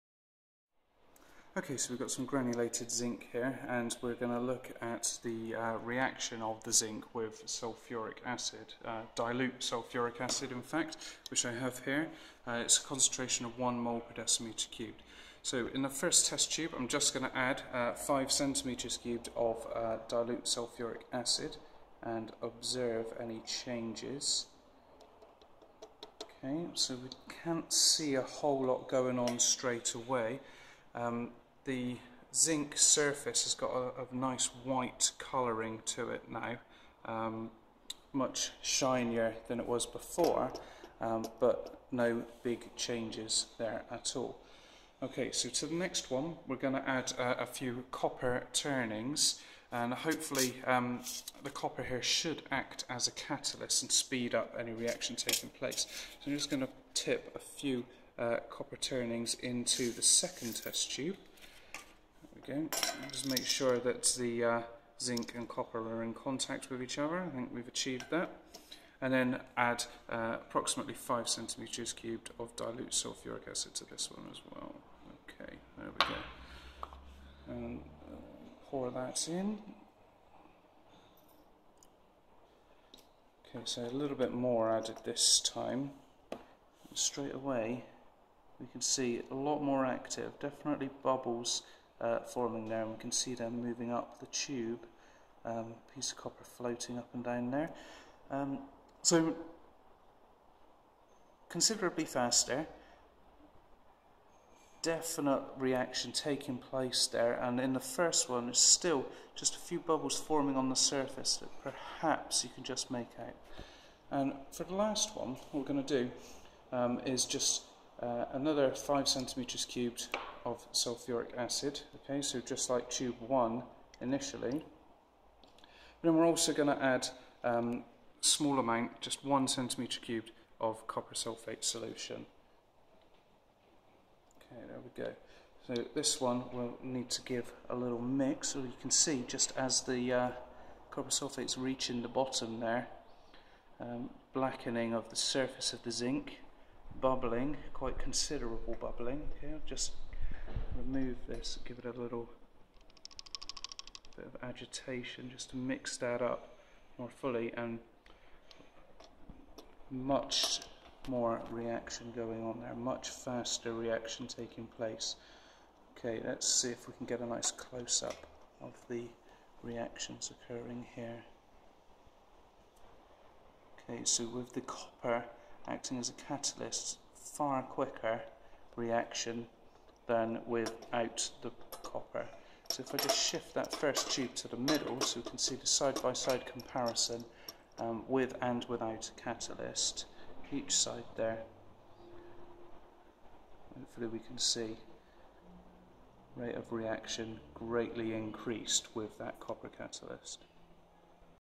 Zinc and sulfuric acid- copper catalyst
Demonstration of the catalytic effect of copper and copper ions on the reaction between zinc and dilute sulfuric acid.